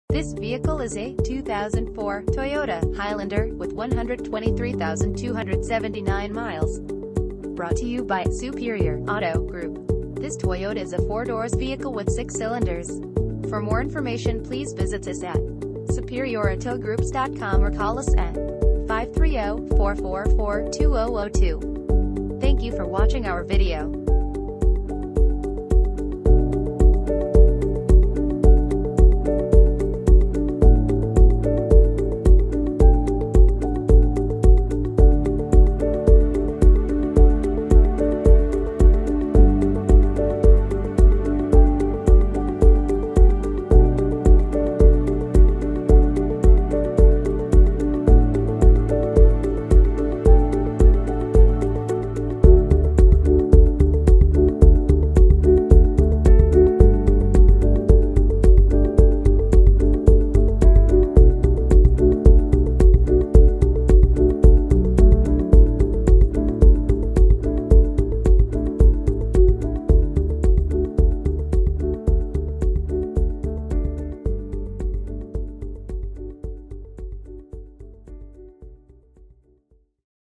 2004 Toyota Highlander - Superior Auto Group
2004 Toyota  Highlander

2 owners, Clean title, 123,279 miles, Automatic, SUV, FWD: Front Wheel Drive, 4 doors, 6 cylinders, Gasoline, MPG City 19 and 25 in Hwy.